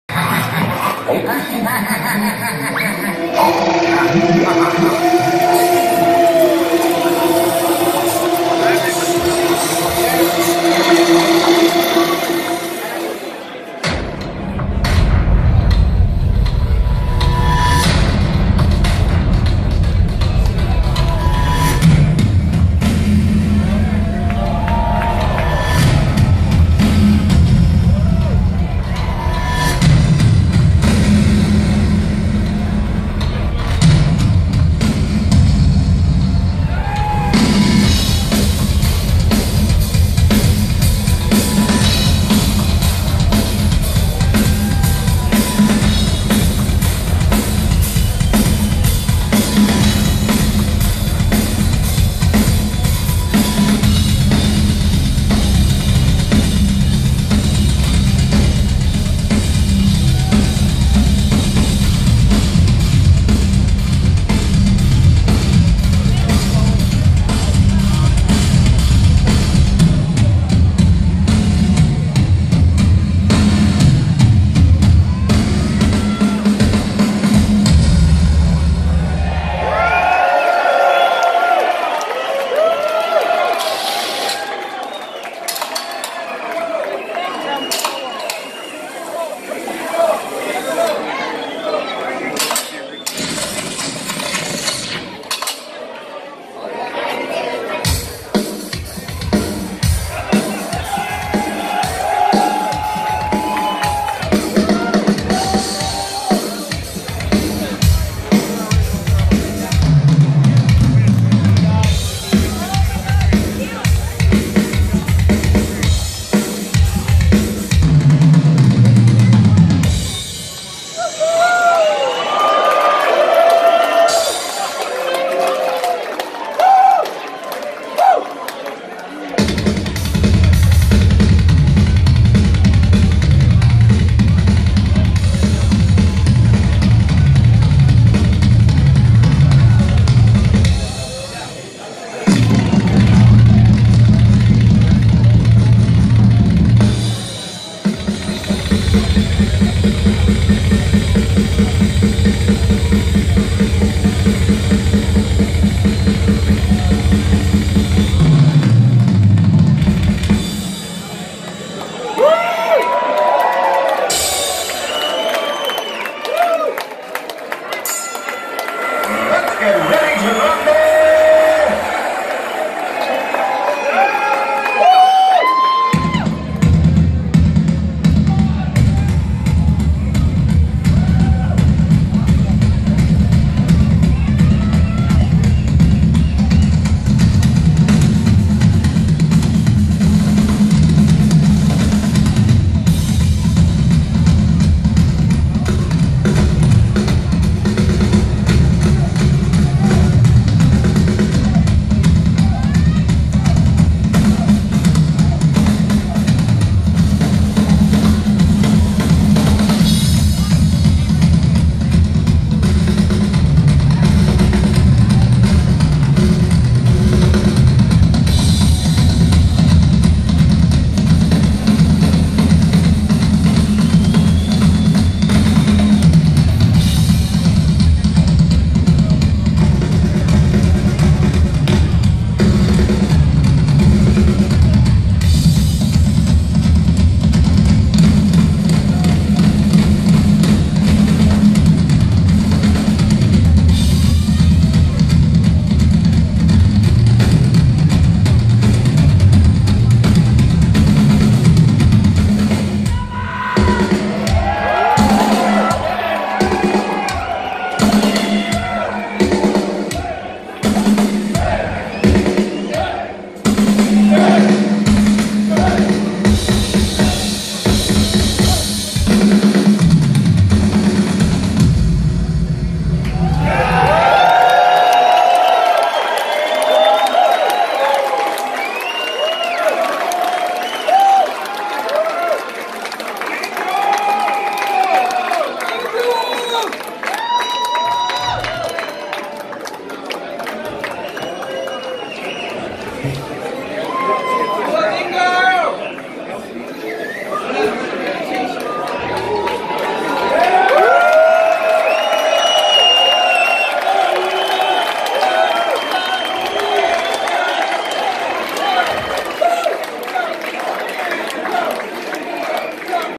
Drum Solo (Dani Löble live & Ingo Schwichtenberg video) HELLOWEEN @ House of Blues, Las Vegas. 2018
Drum Solo (Dani Löble live & Ingo Schwichtenberg recorded video) HELLOWEEN @ House of Blues, Las Vegas. Pumpkins United World Tour 2017/2018